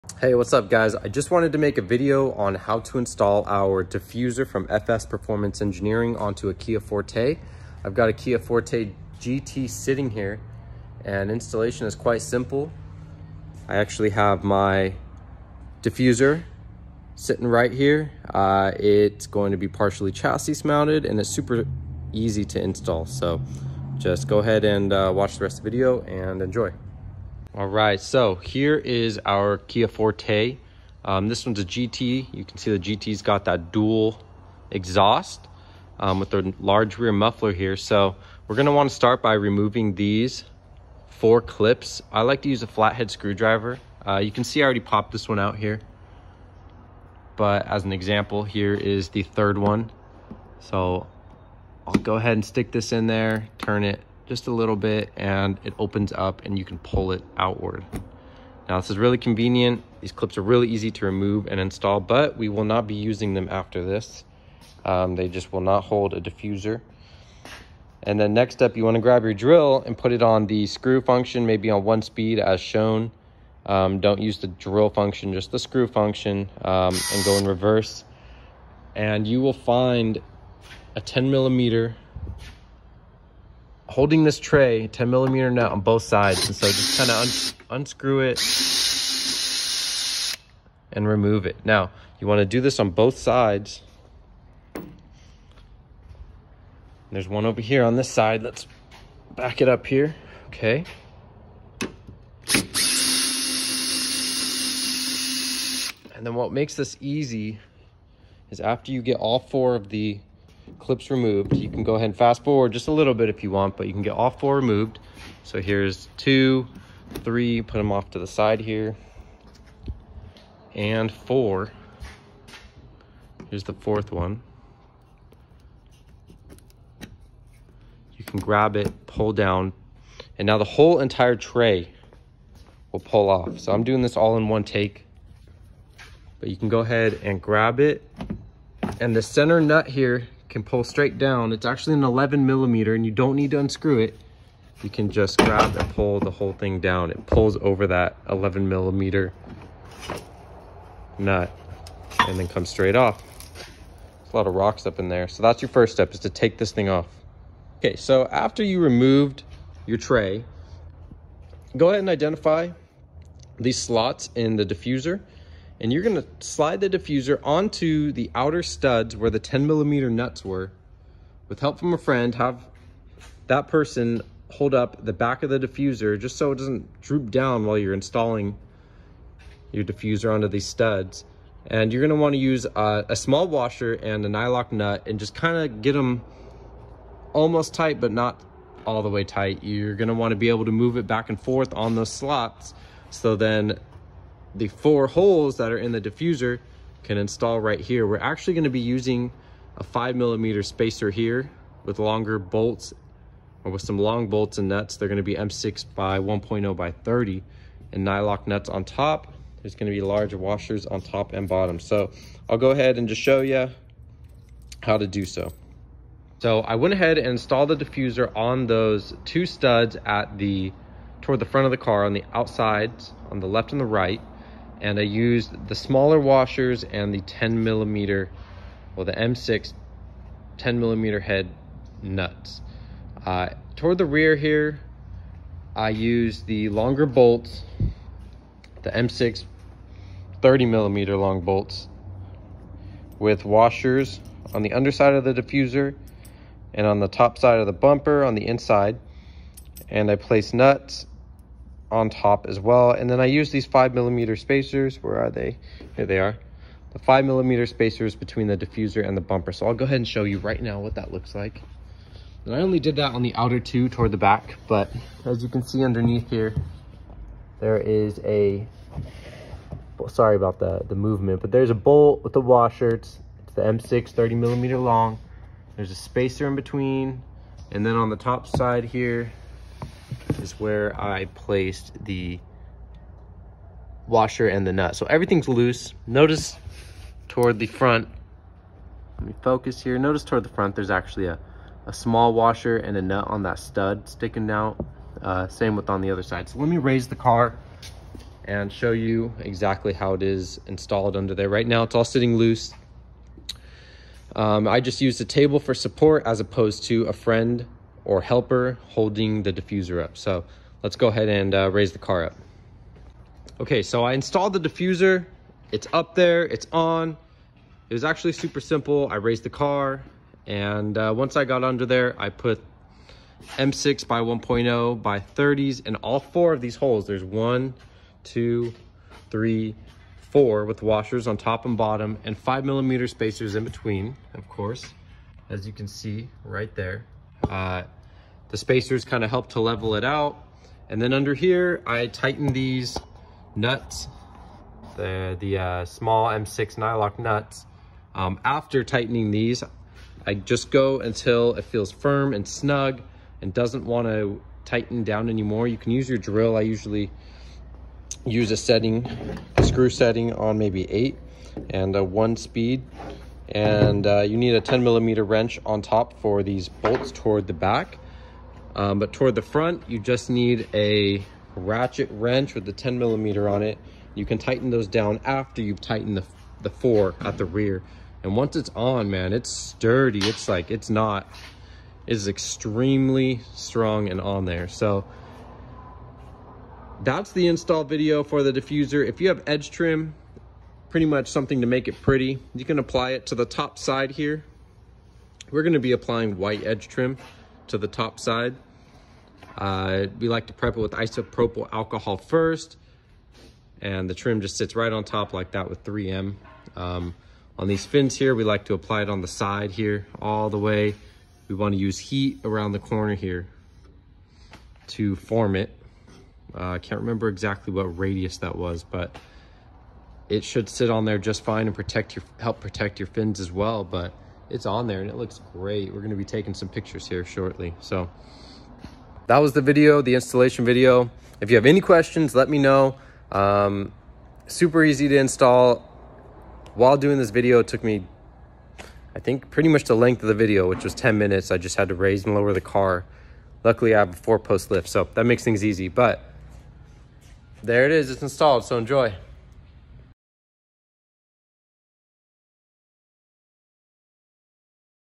Kia Forte GT (2019-2021) Rear Diffuser Installation by FSPE Inc.
FS Performance Engineering has designed a rear diffuser that mounts to your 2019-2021 Kia Forte GT without any drilling.

Does not fit the GT-Line or Base Model.

Supplied hardware includes:
(2) 1/4” ID Small Washers
(4) M6-1.0 x 30MM Bolts
(6) M6-1.0 Nyloc Nuts
(8) 1/4” ID, 1” OD Washers

Required tools for installation:
- Flat head screwdriver
- 10mm socket
- 10mm wrench
- Small ratchet wrench

Optional tools for faster installation:
- Drill with socket attachment